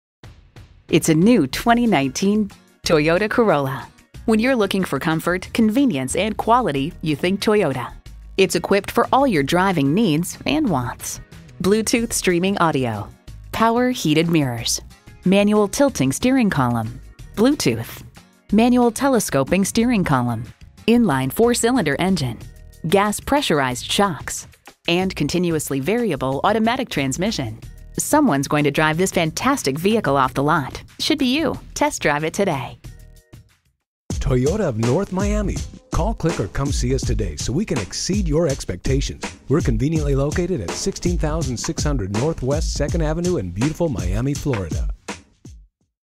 2019 Toyota Corolla Miami FL Ft Lauderdale, FL
Year: 2019
Make: Toyota
Model: Corolla
Trim: LE
Engine: 1.8L I4 DOHC Dual VVT-i
Transmission: CVT
Color: Red
Stock #: C923695
VIN: 5YFBURHE3KP923695

Address:
444 NW 165th Street